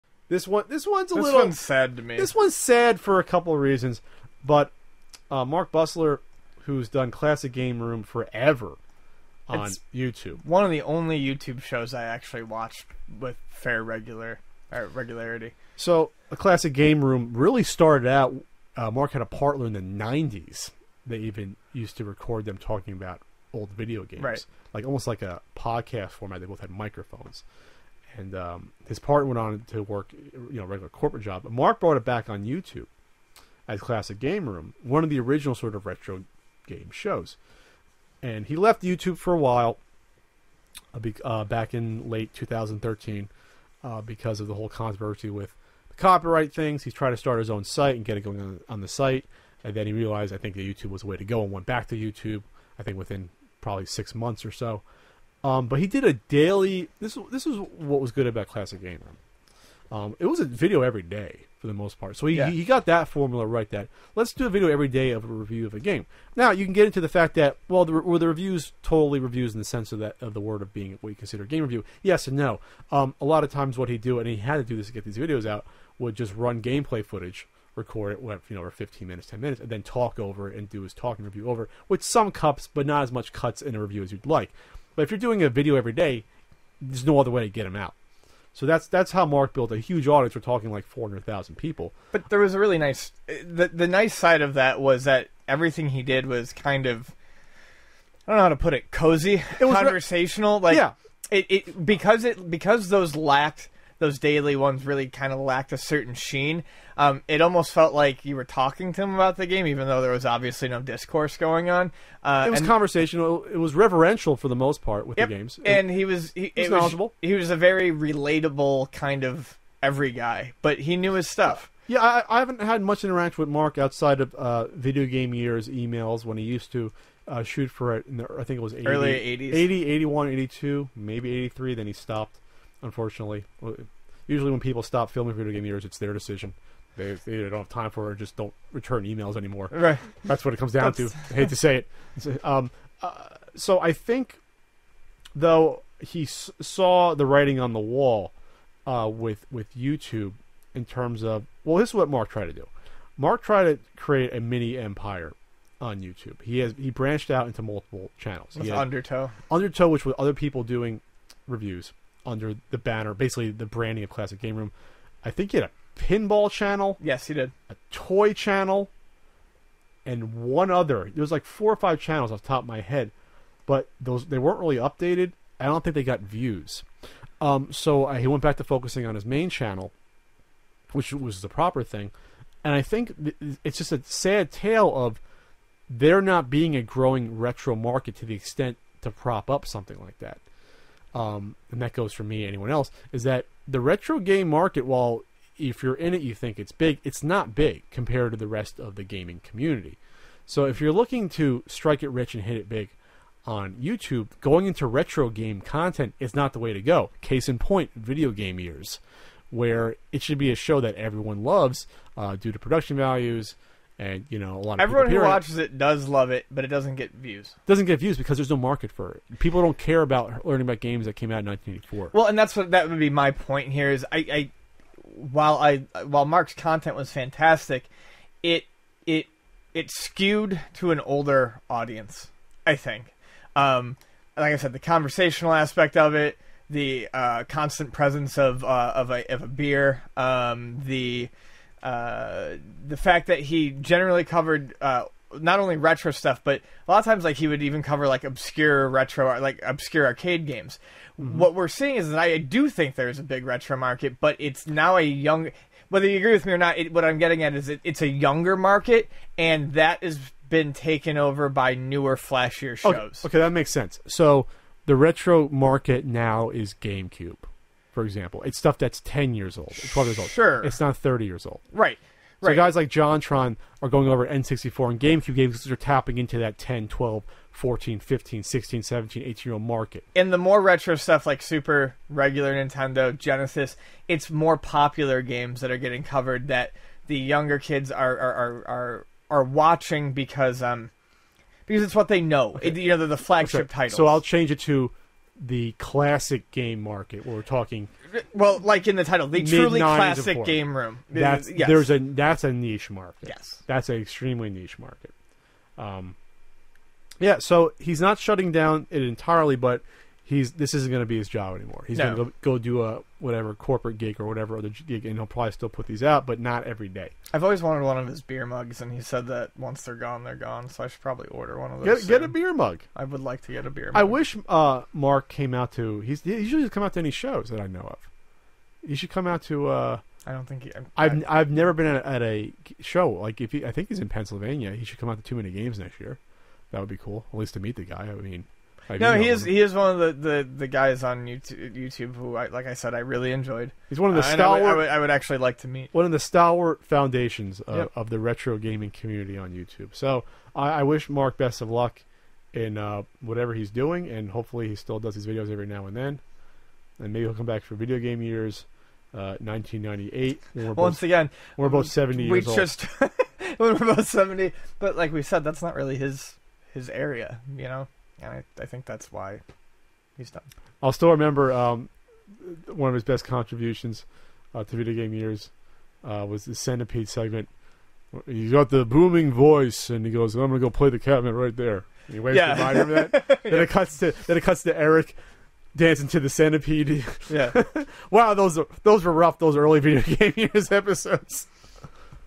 Classic Game Room Show Ending?
Classic Game Room in its current form may be ending, with creator Mark Bussler moving on.